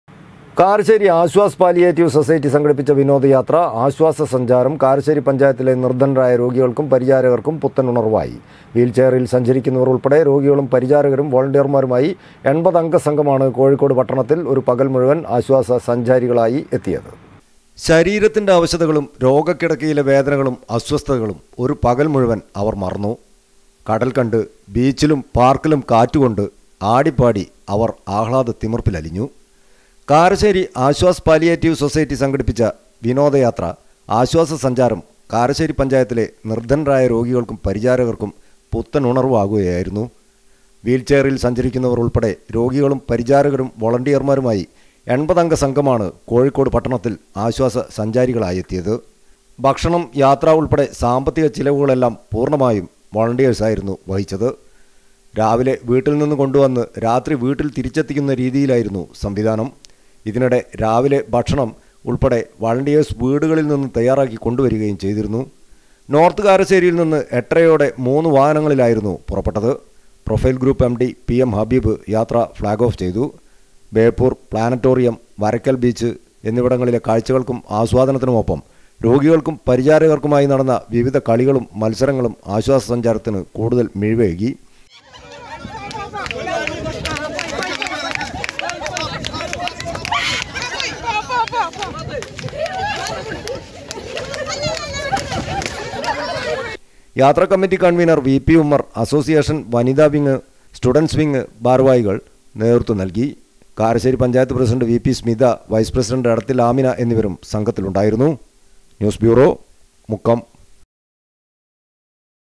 കാരശ്ശേരി ആശ്വാസ് പാലിയേറ്റീവ് സൊസൈറ്റി സംഘടിപ്പിച്ച വിനോദയാത്ര' ആശ്വാസ സഞ്ചാരം "
കാരശ്ശേരി ആശ്വാസ് പാലിയേറ്റീവ്  സൊസൈറ്റി  സംഘടിപ്പിച്ച വിനോദയാത്ര' ആശ്വാസ സഞ്ചാരം " കാരശ്ശേരി പഞ്ചായത്തിലെ നിർധനരായ രോഗികൾക്കും  പരിചാരകർക്കും പുത്തനുണർവായി.